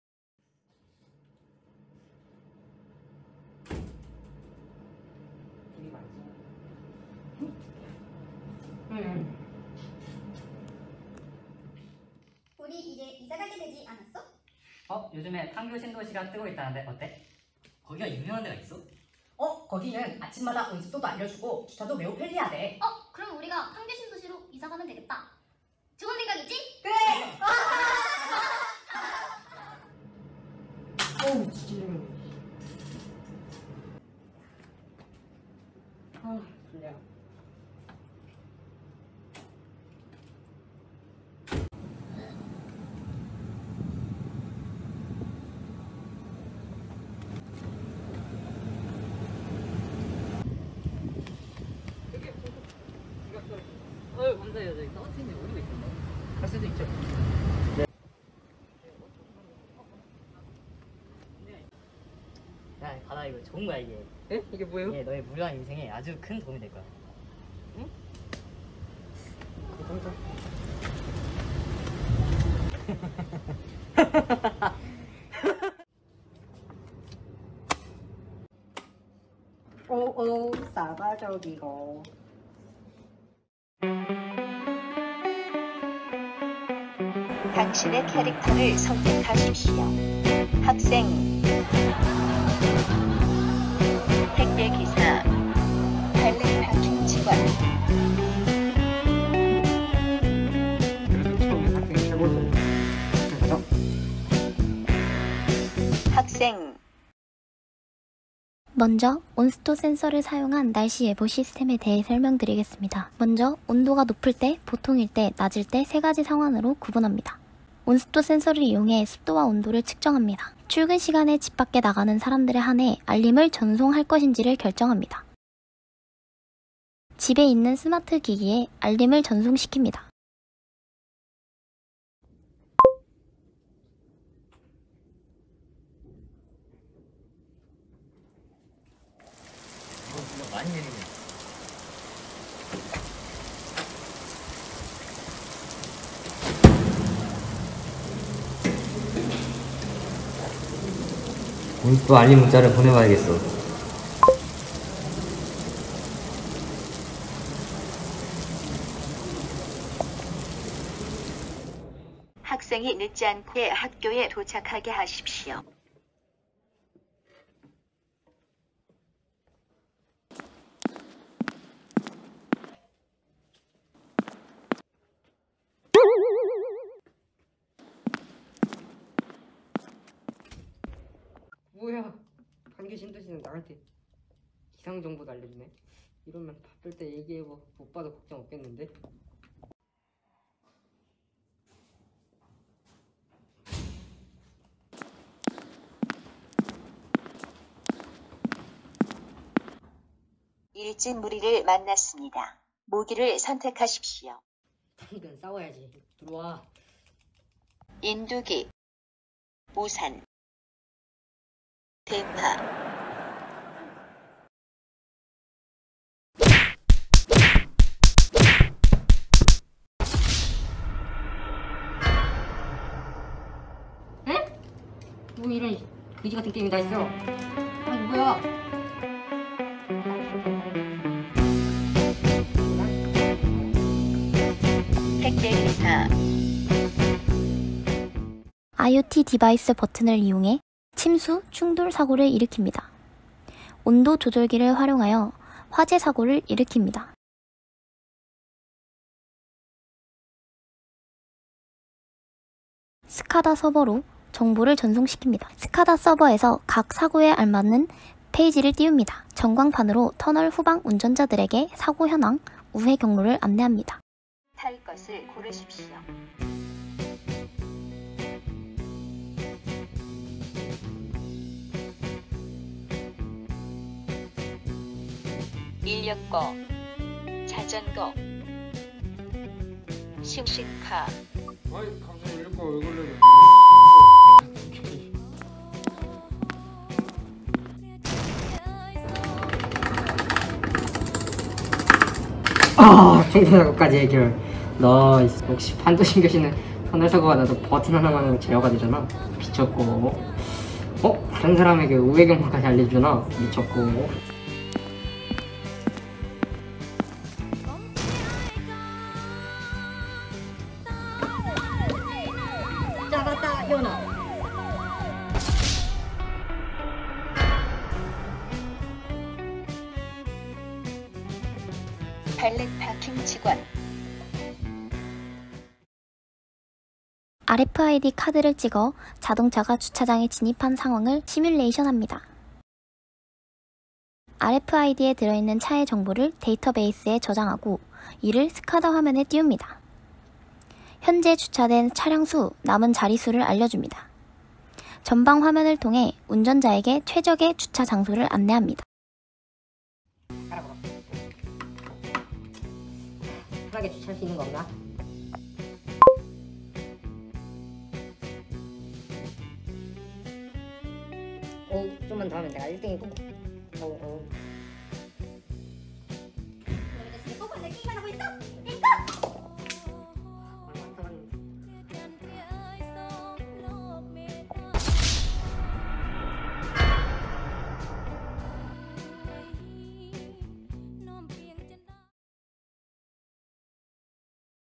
수도전기공업고등학교 에너지전자제어과 6조(스마트 시티)
2020학년도 프로젝트 기반 학습 결과물
수도전기공업고등학교 에너지전자제어과 5조
SCADA를 활용한 산업용 스마트 제어시스템 만들기
스마트 시티(Smart City)